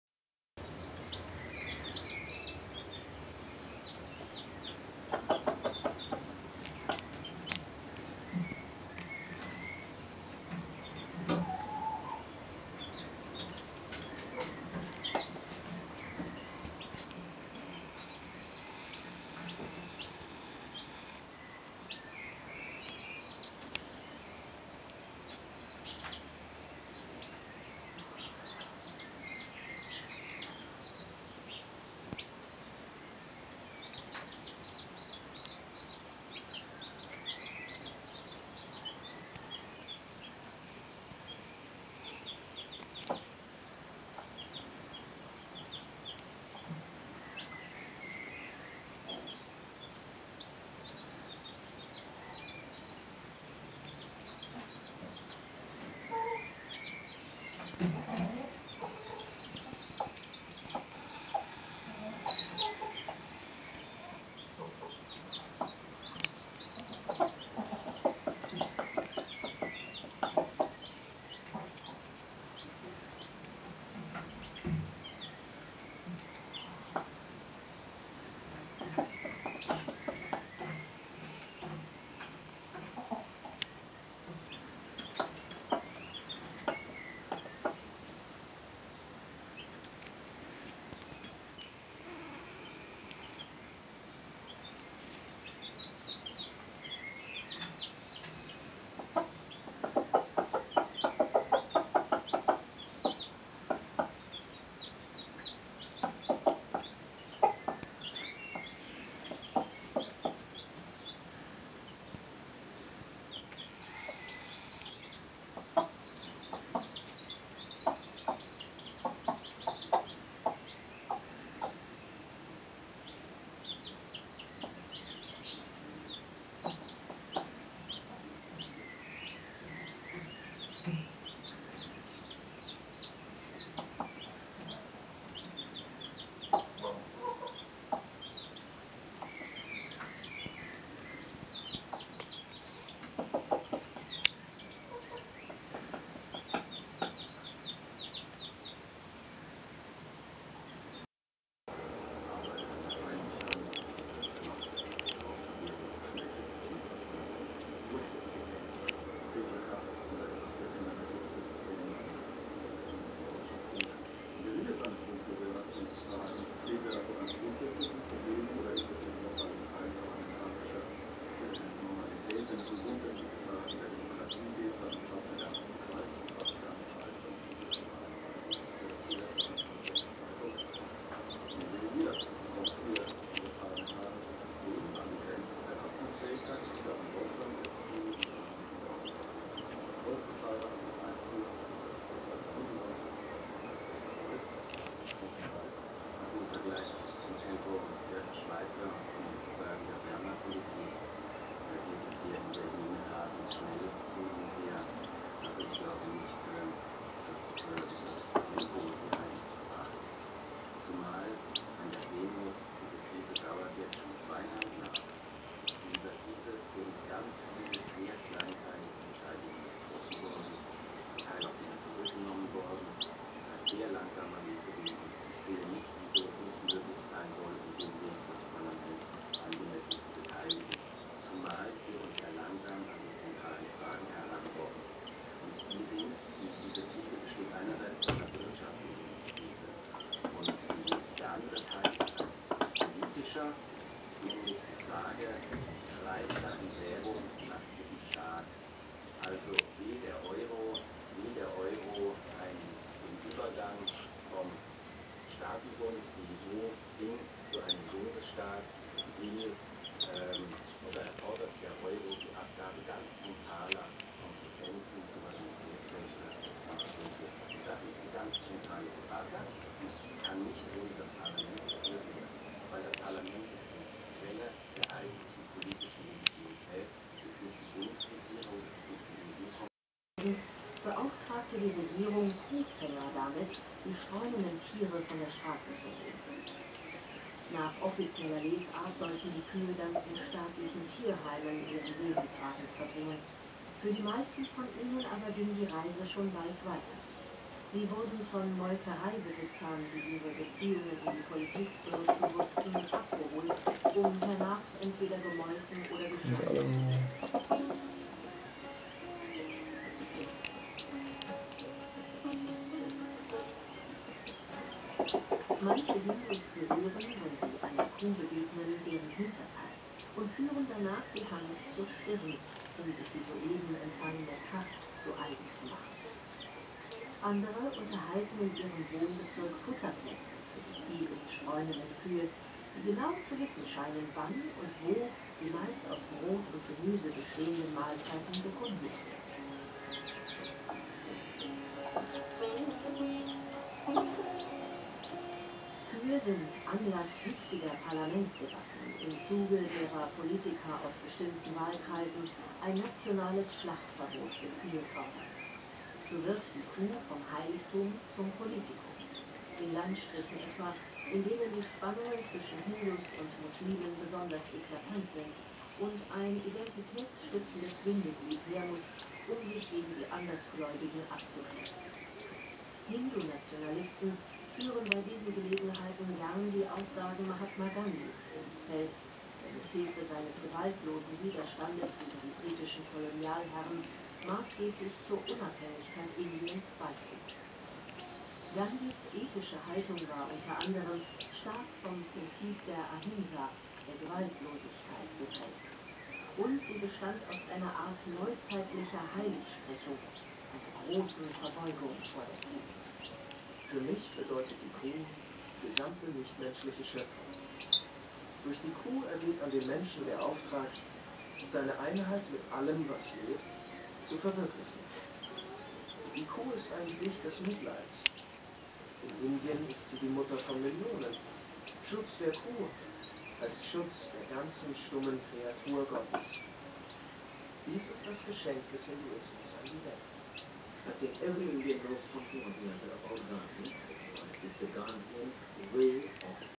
Glucke und Küken
Glucke (Vorwerk) mit Küken (Vorwerk dunkel, Sundheimer hell) - einen Tag alt
(am 21. Bruttag)